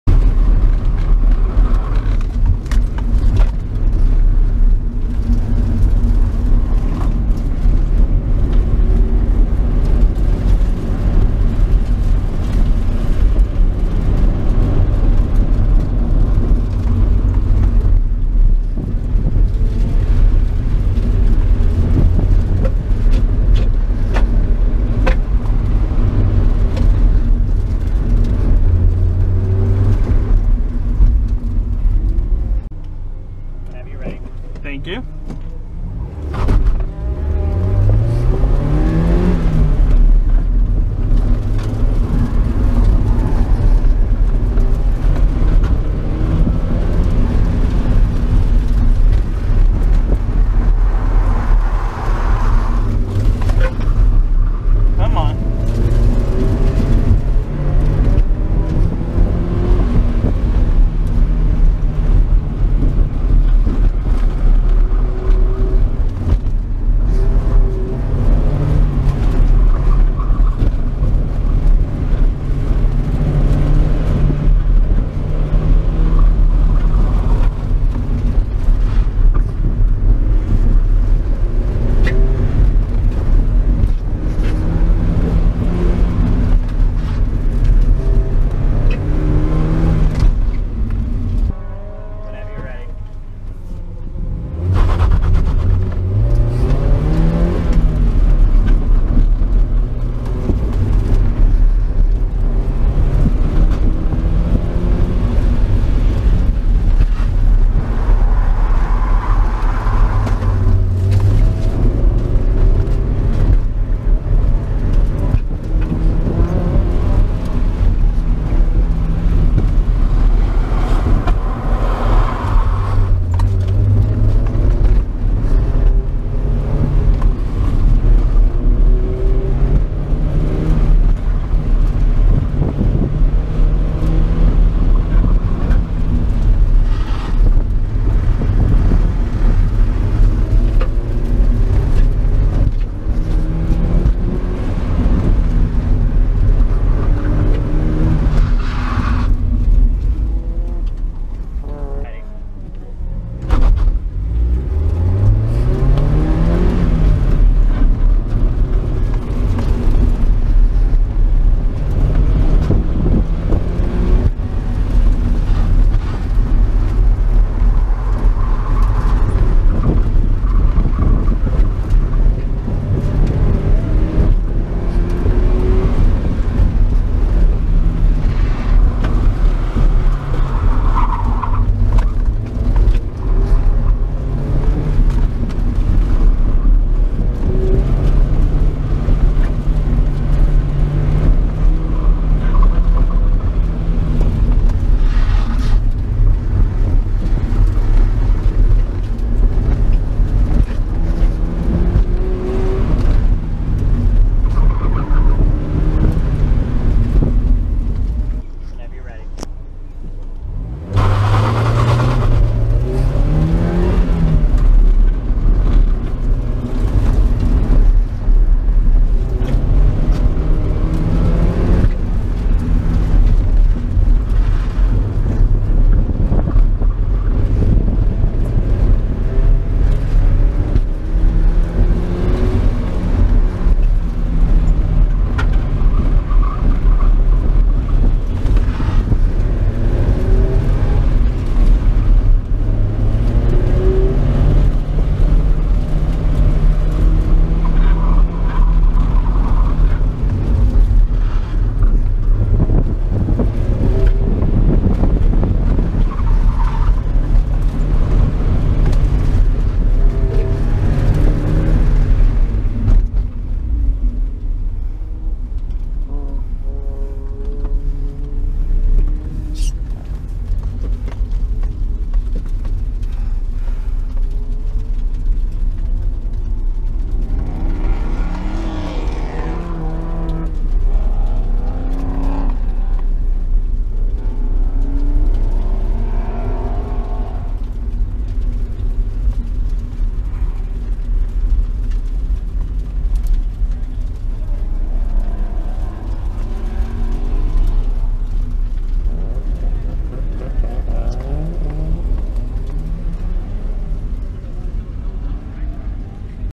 Solo NYR 4/23/23
Using oversized tires on the car but they are a little old. Took me a while to get some confidence in them, also only filmed about 4 runs of 8 total runs, ops.